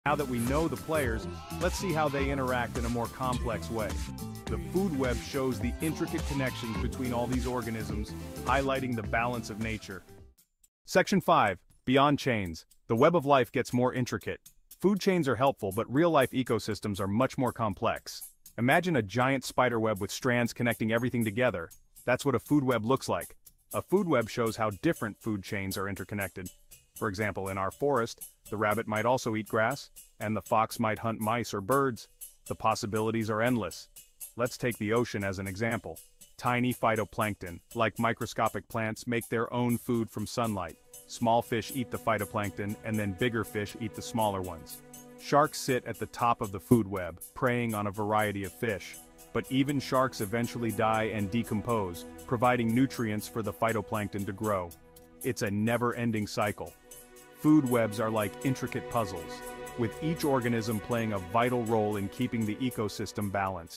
Food Web: Unveiling Nature's Intricate Connections
The food web shows the complex relationships between organisms, highlighting nature's balance. Discover how different food chains interconnect, like a giant spider web. Each organism plays a vital role in keeping the ecosystem balanced.